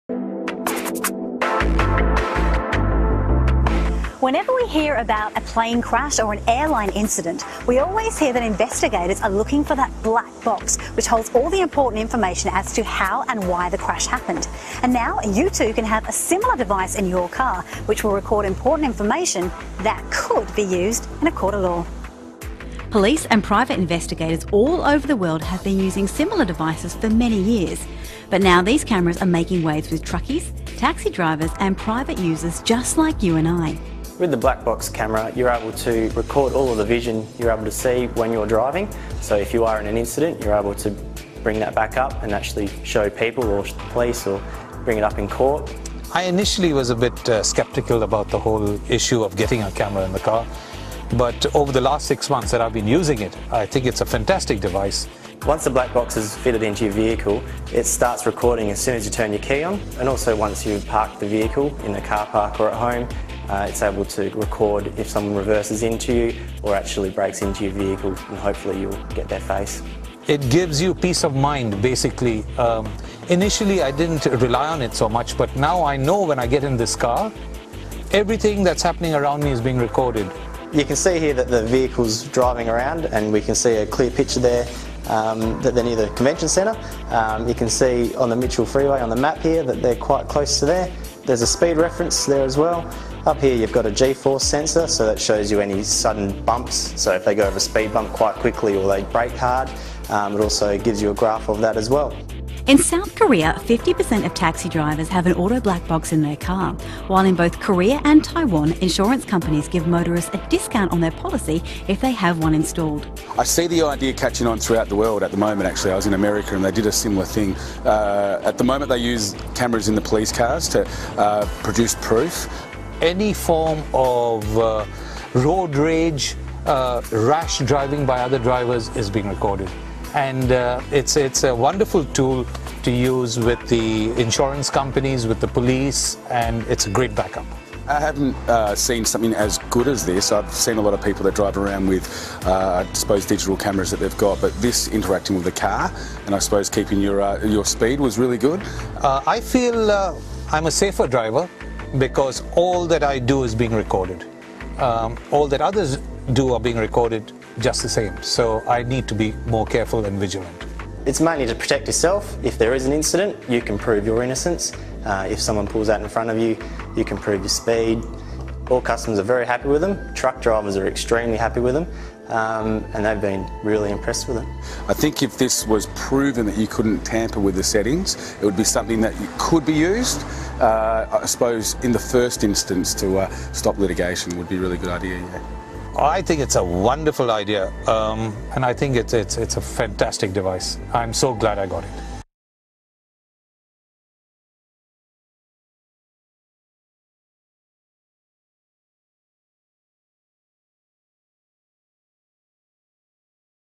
The importance of Black Vue Vehicle Black Box
BlackVUE, why is it so important to have a BlackVUE camera installed in your vehicle?